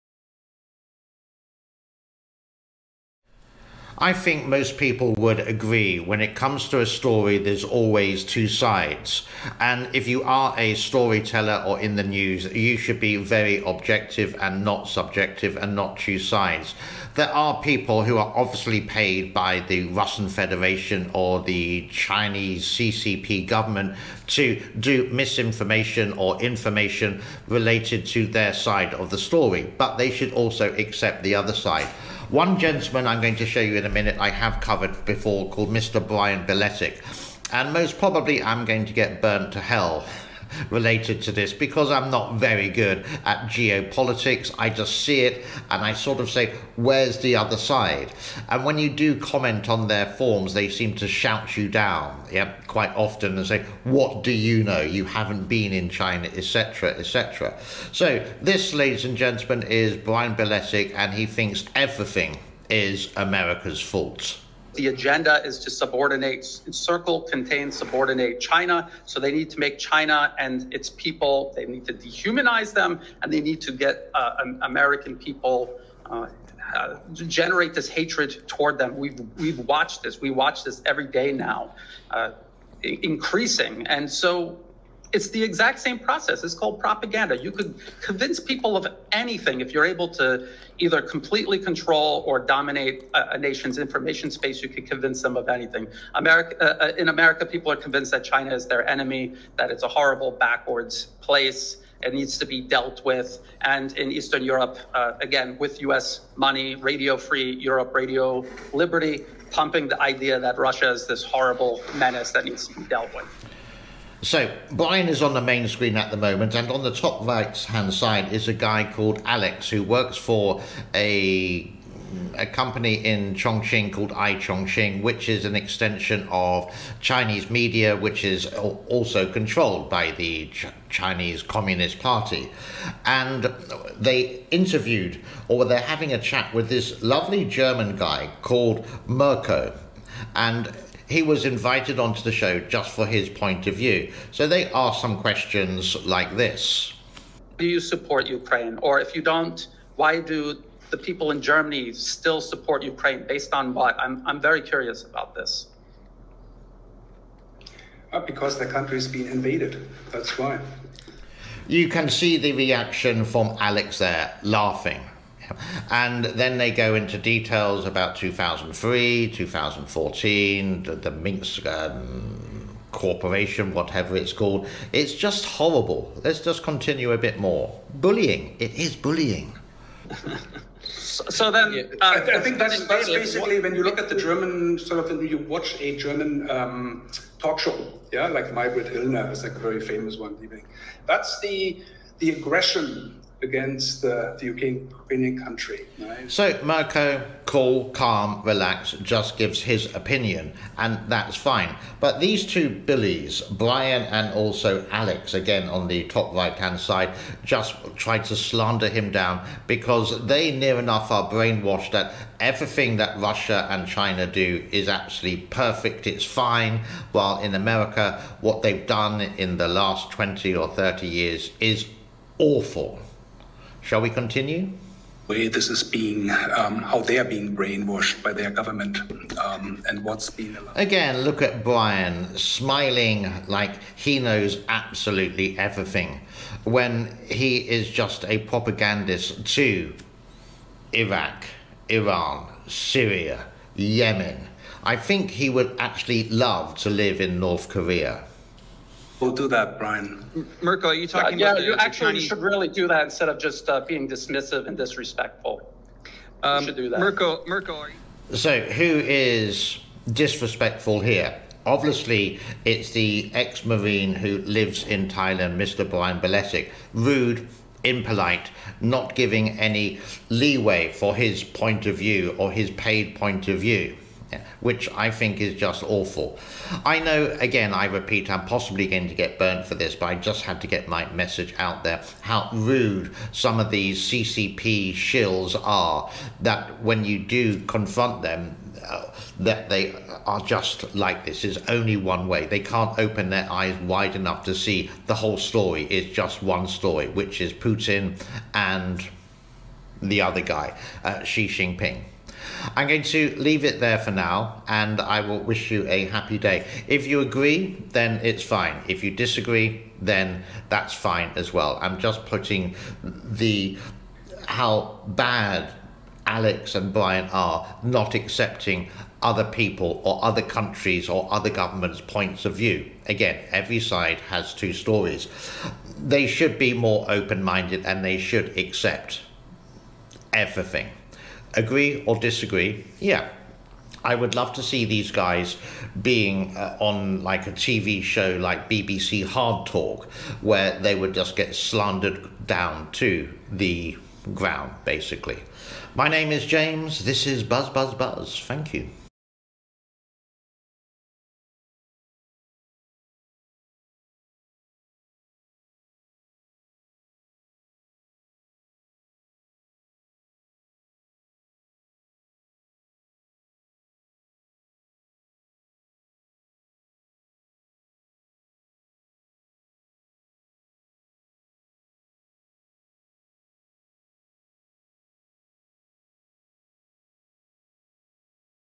Two Anti West Bullies - Hit Hard on a German
Here are 2 bullies who seem to hate the USA and blame the USA for everything on the USA.
Here they are like wolves attacking this German Guy.

Check these channels out.